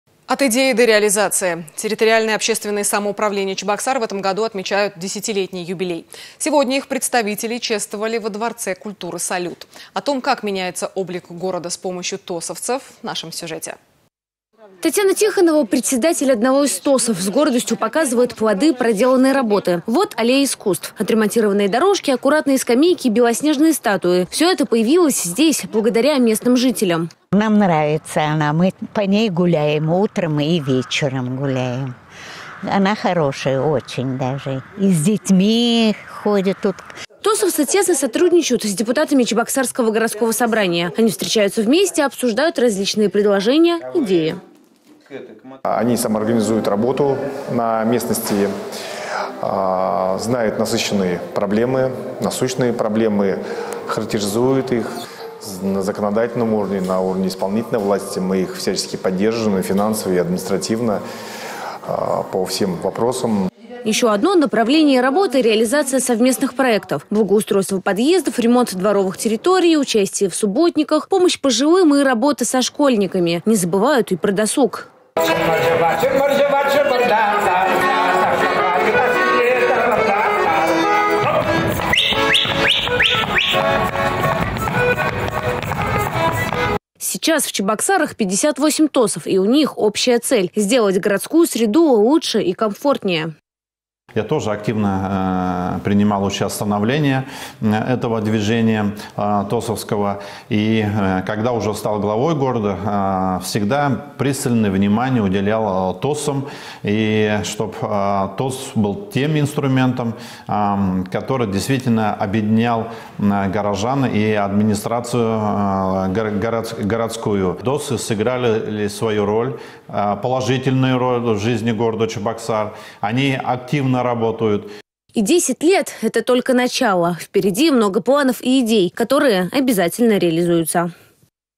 Территориальные общественные самоуправления Чебоксар в этом году отмечают 10-летний юбилей.
От идеи до реализации. Территориальные общественные самоуправления Чебоксар в этом году отмечают 10-летний юбилей. Сегодня их представителей чествовали во дворце культуры Салют. О том, как меняется облик города с помощью тосовцев, в нашем сюжете